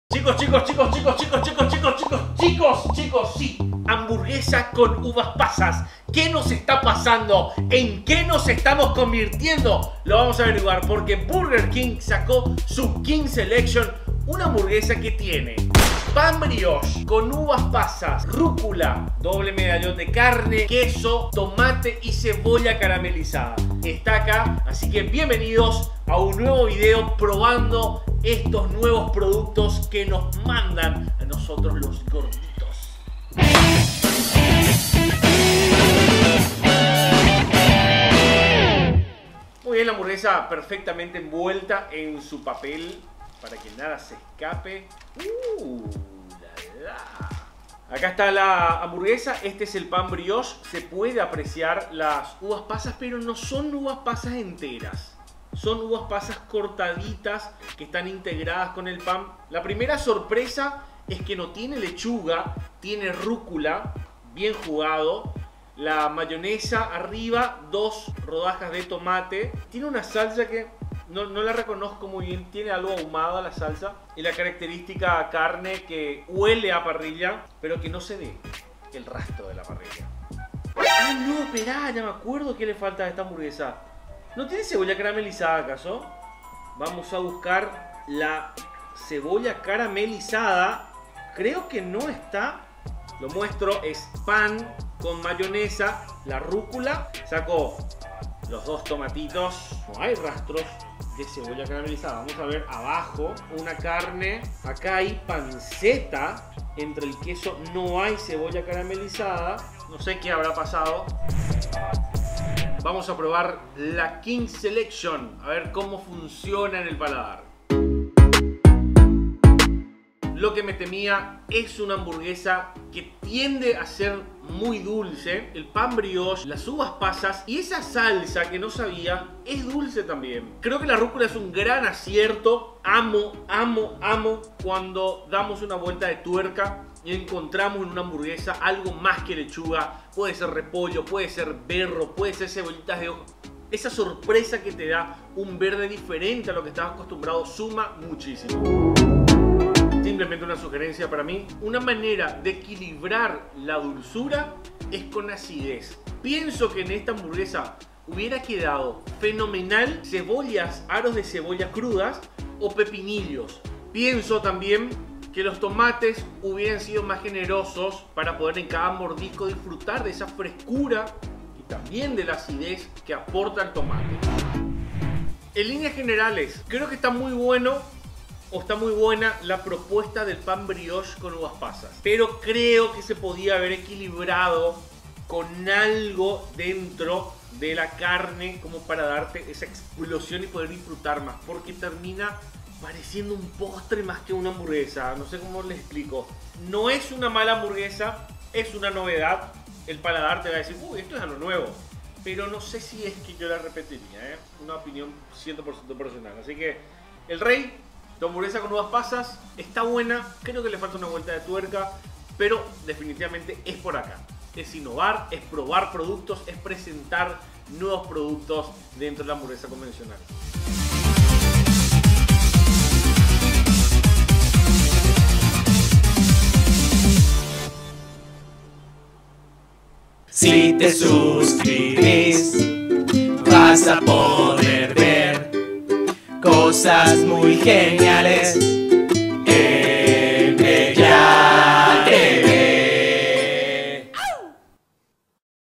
PROBANDO LA HAMBURGUESA CON UVAS PASAS DE BURGER KING
Pan Brioche, Uvas pasas, doble carne, rúcula y... ¿cebolla caramelizada?